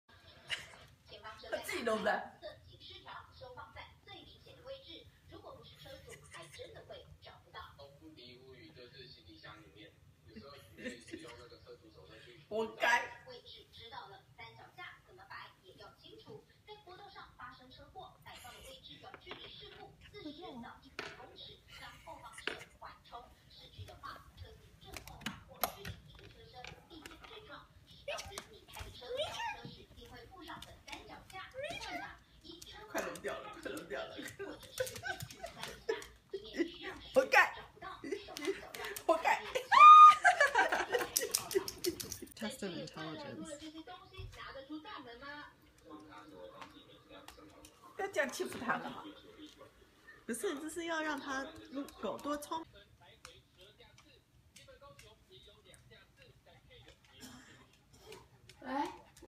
Major the Schnoodle's blanket test
Just wanted to see if Major knew how to escape from a fuzzy blanket.